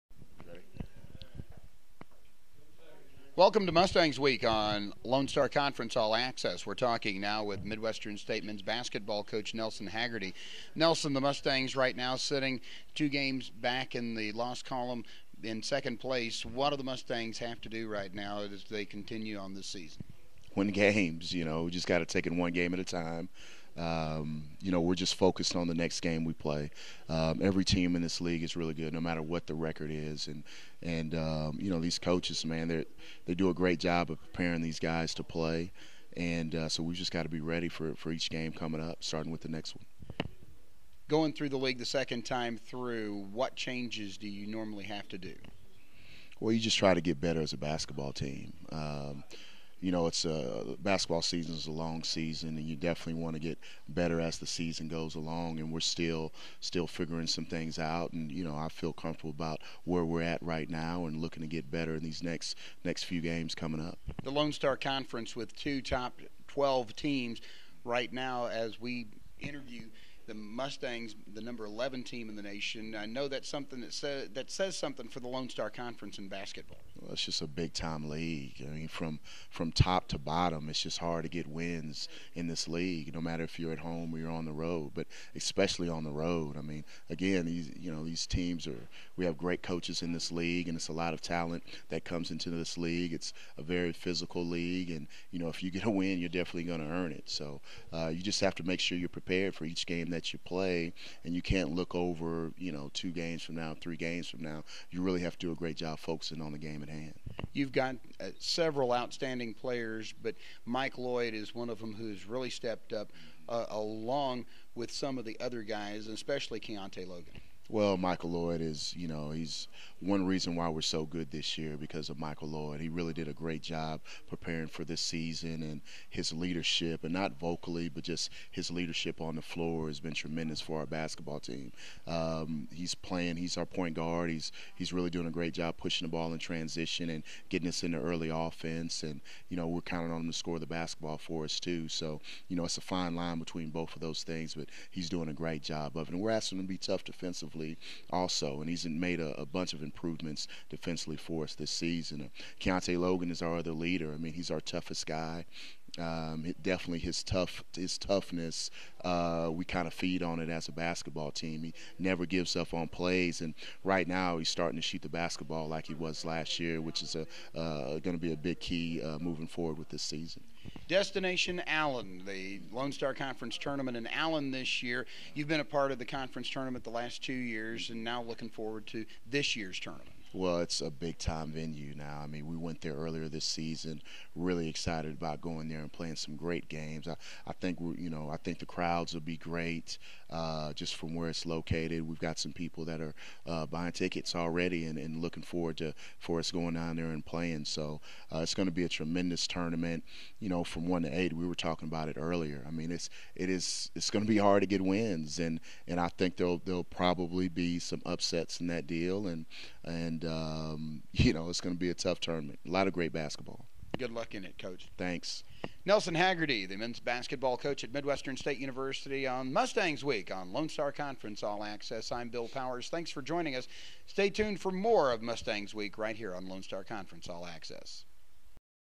Mustangs Week: Nelson Haggerty, men's basketball coach
Midwestern State men's basketball coach Nelson Haggerty speaks with assistant athletic director Bill Powers for Mustangs Week.